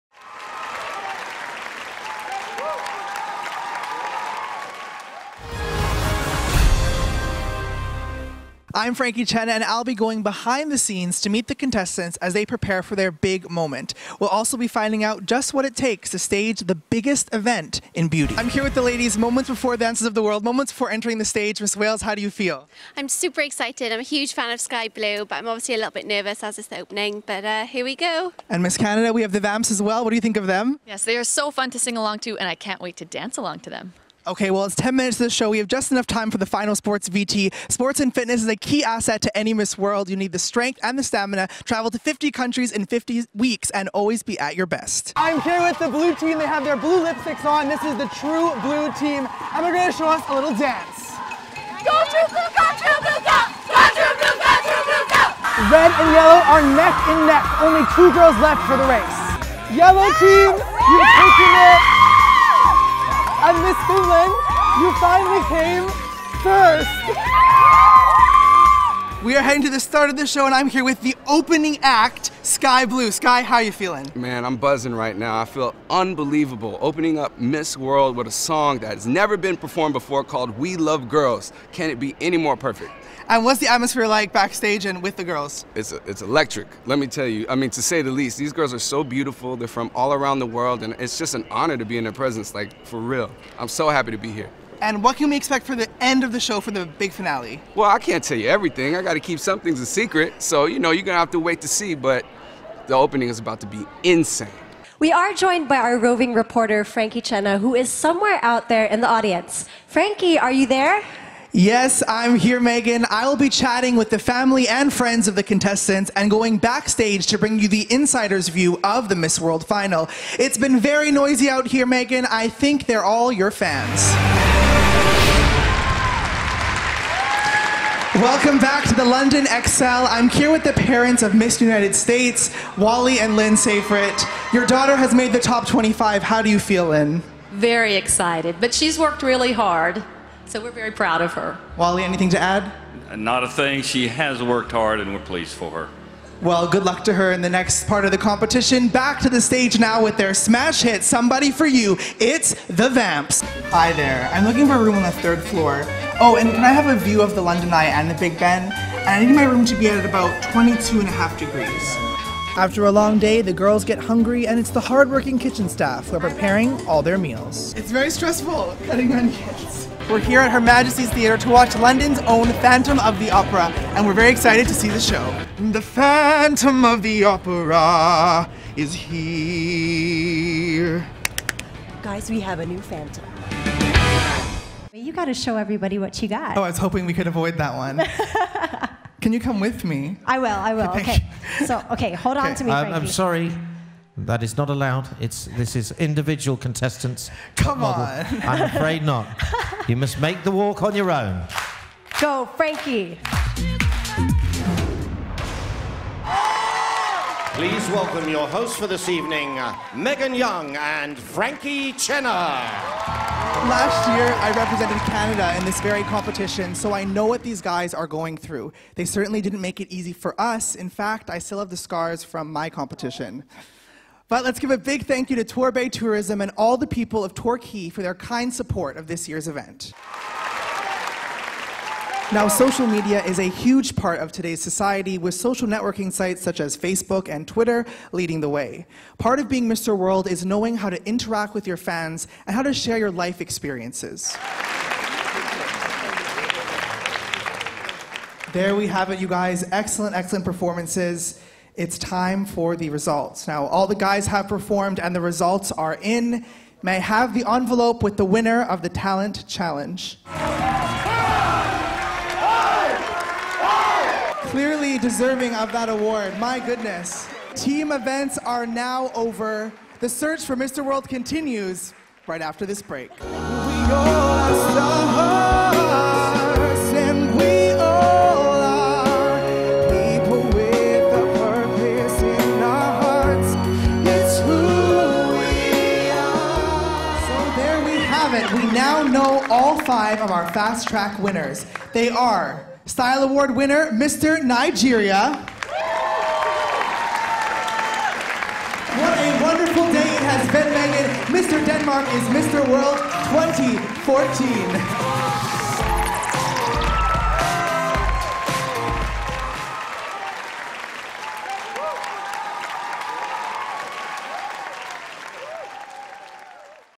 Frankie Cena Miss World & Mr World 2014 Presenter Reel
Look back on the journey of Miss World Web Presenter and Mr World Presenter, Frankie Cena as he hosted both programs in 2014.

Miss World 2014- London, England
Mr World 2014- Torquay, England